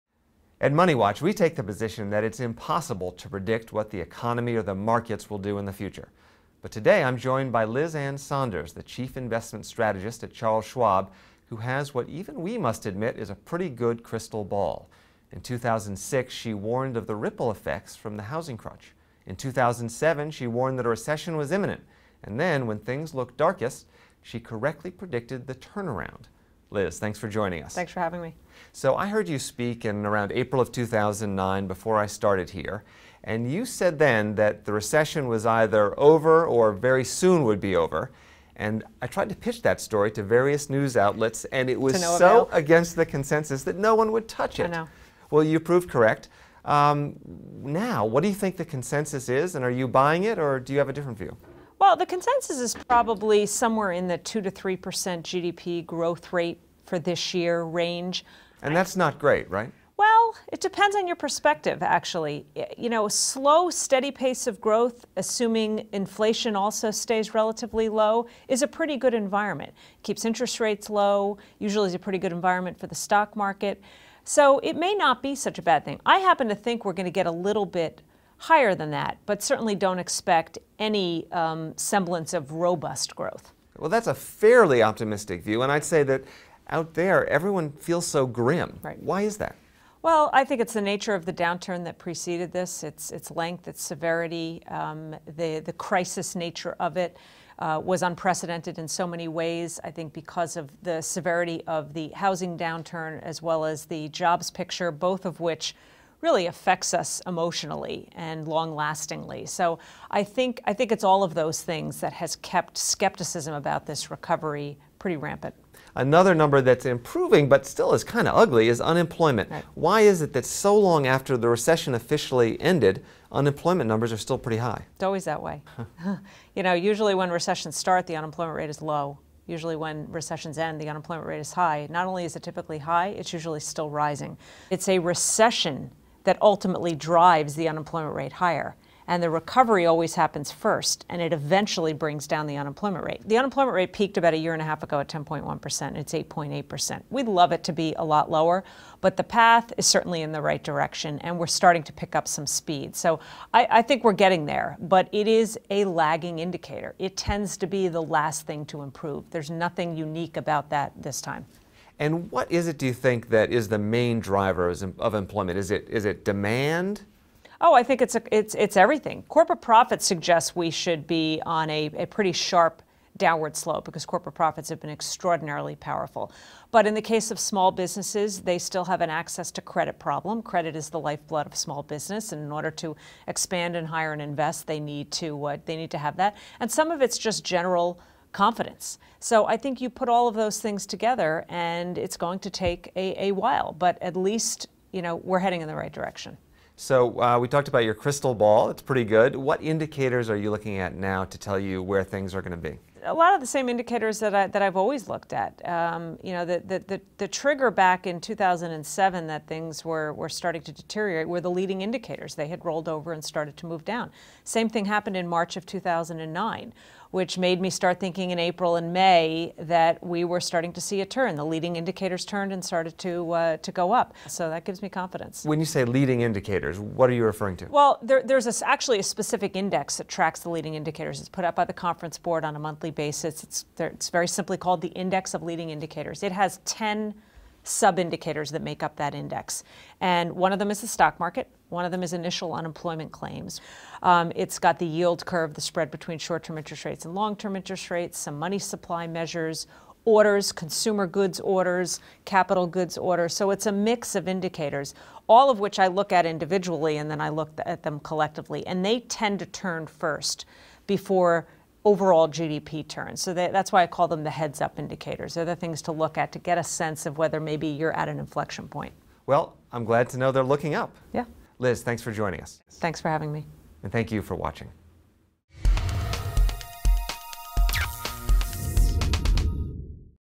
Reading the Economic Indicators
Chief Investment Strategist for Charles Schwab, Liz Ann Sonders, makes sense of the Index of Leading Indicators predicting a steady growth rate of 2-3%, with low inflation. Sonders sits down with MoneyWatch.com's Executive Editor, Jack Otter.